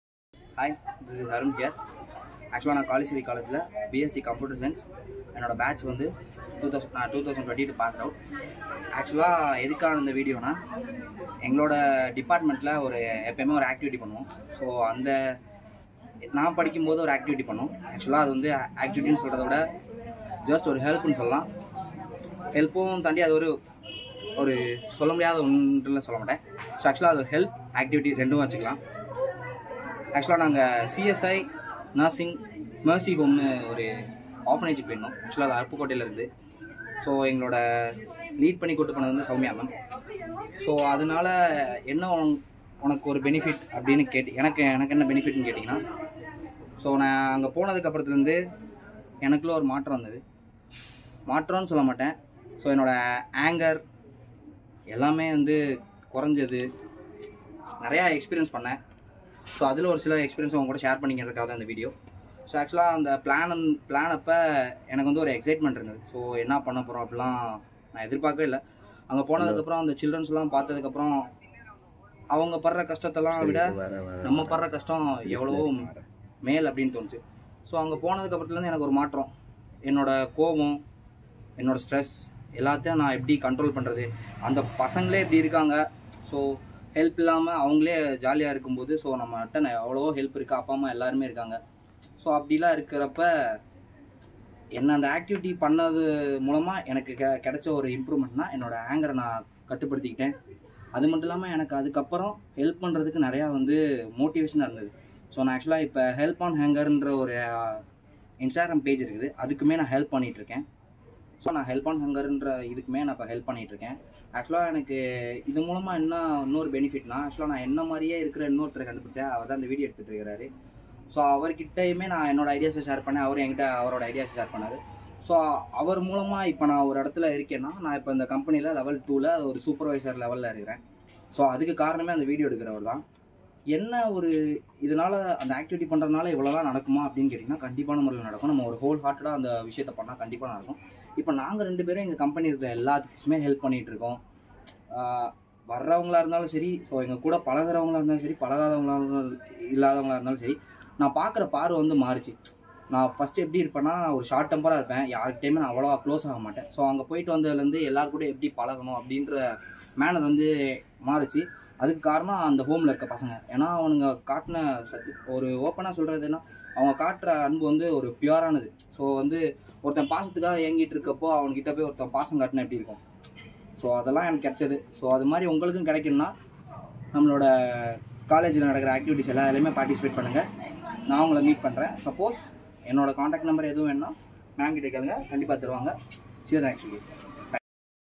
Arun – CS | KUSHI: (Sri) Kaliswari’s Unique “Spreading Happiness"
His wholehearted regular contribution to the Help on Hunger Instagram page is a token of evidence of the attitudinal transformative experience he had after his volunteering visits to CSI Orphanage home,  Aruppukottai.